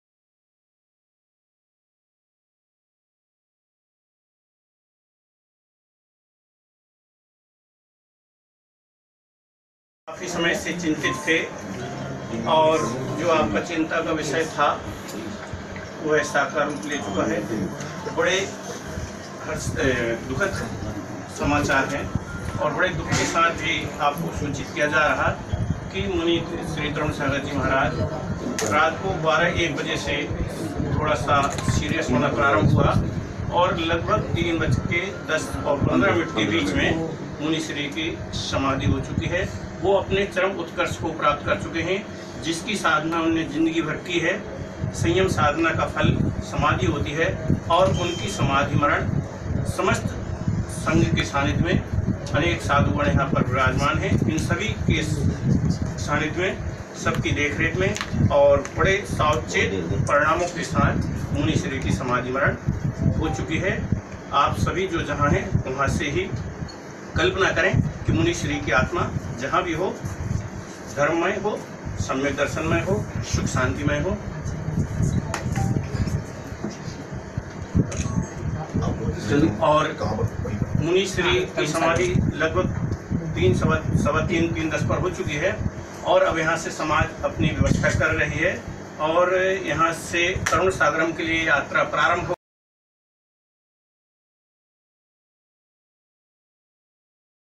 Jain Muni Tarun Sagar Samadhi - Jain Monasticism
Video Uploaded by Sonam Jain

क्रांतिकारी राष्ट्रसंत मुनिश्री  तरुणसागरजी की समाधी सल्लेखना पूर्वक अभी 1 सितंबर 2018 को प्रातः 3.18 को

मुनिश्री  अरुणसागरजी ससंघ
आचार्यश्री  सौभाग्यसागरजी
उपाध्याय गुप्तिसागरजी
मुनिश्री अनुमानसागरजी
मुनिश्री शिवानंदसागरजी ससंघ
 साधुओं के सानिध्य में संपन्न हुई।

अंतिम संस्कार विधी आज दोपहर 3 बजे
तरुणसागरम तीर्थ
दिल्ली मेरठ हायवे,  दिल्ली स्टेशन से 25 किमी दूर पर होगा।

यात्रा सुबह 7 बजे राधेपुरी दिल्ली से प्रारंभ होकर 28 किमी दूर तरुणसागरम पर पहूंचेगी..
कृपया आप एक बार नवकार मंत्र जरूर बोले हमारे महाराज जी तरुण सागर जी के लिये